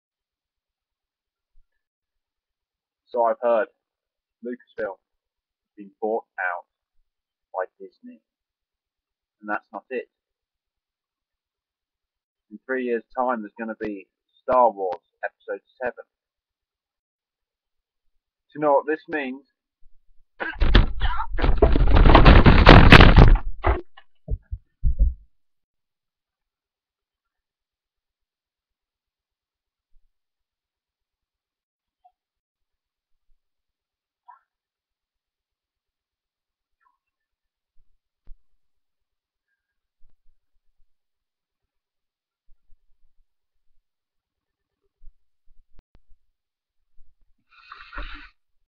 My reaction to STAR WARS EPISODE VII
Just found out that LucasFilm has been bought by Disney resulting in the long-awaited, long-rumoured sequel trilogy (VII, VIII, IX)

Here's my thoughts on the matter...